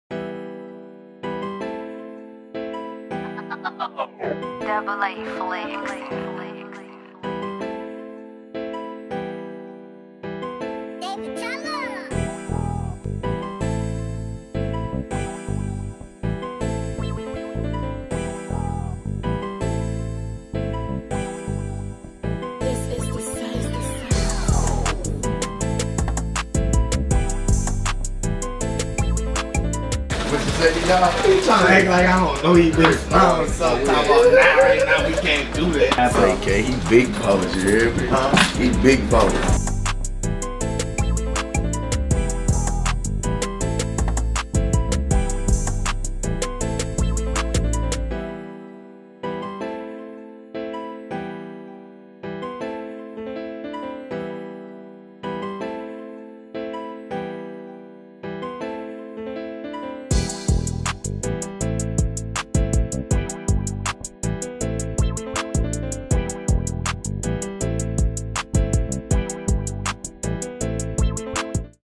NBA YOUNGBOY Brings 100 Thousand Cash to ICEBOX!!!
Louisiana's NBA YOUNGBOY stops by Icebox and puts $100,000 CASH on the table!! He also treats himself to a new spike bracelet!!

WATCH NOW!

Icebox Diamonds & Watches
3255 Peachtree Road NE